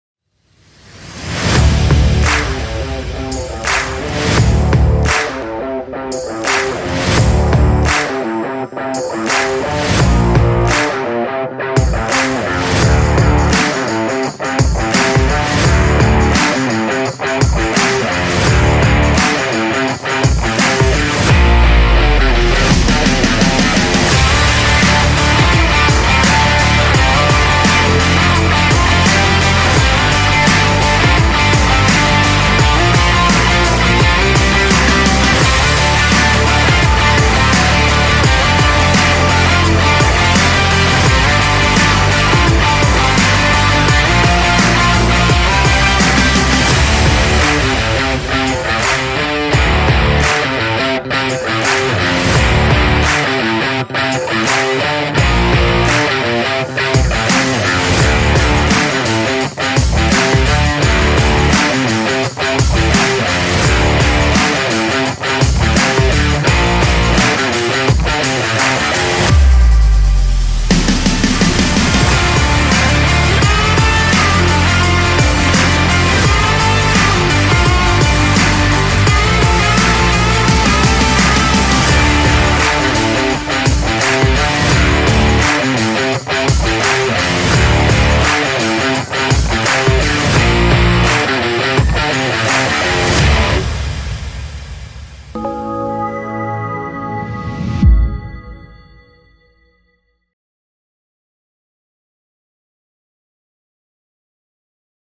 PSCWMAF2022 | DAY 6 - Kickboxing Highlights
Join our countdown for TEAMPHIinAIMAG2023 as we end our 8th Women's Martial Arts Festival with the highlights in Kickboxing.